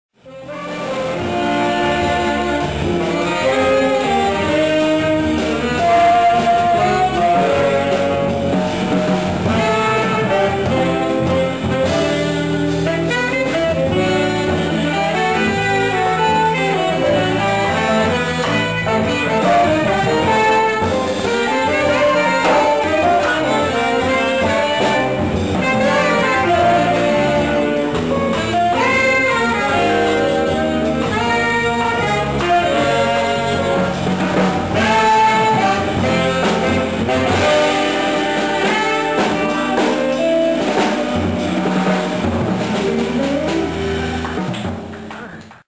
Rob Haight, Robin Boudreaux, Nate Heffron
Three Saxes at once! Robin Boudreaux, Rob Haight and on the far right Nate Heffron. Also Ric Harris on guitar. This LIVE MUSIC event was the Jazz and Jam Session hosted by the Patrick Terbrack Quartet at Cafe Galeria, 5130 N. Western, Chicago, IL.

For more info about LIVE MUSIC EVENTS of all kinds in CHICAGO